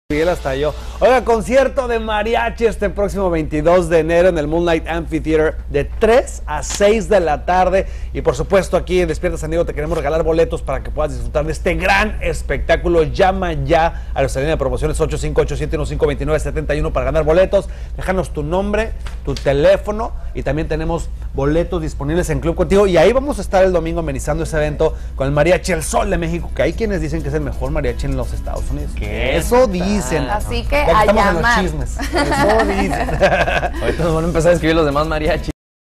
Concierto del Mariachi
proximo 22 de enero en el moonlight amphitheatre...de 3 a 6 de la tardes..y despierta san diego te quiere regalar boletos para que puedas disfrutar de este gran espectaculola llama a nuestra linea de promociones (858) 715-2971para ganar boletos...dejanos tu nombre y telefono...tambien habran boletos disponibles en el club contigo